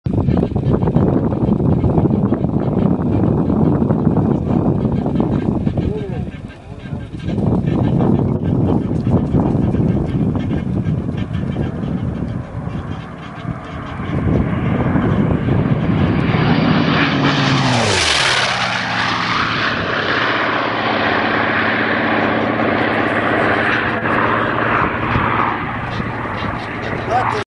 F4U-5N visits Pappy Boyington Field, Cd'A, ID
In honor of the Marines and Pappy Boyington, born here in Coeur d'Alene ID, a Corsair was flown over from Kalispell MT. He flew in and buzzed us at about 300knts and this was his departing fly by which seemed like 300+knts.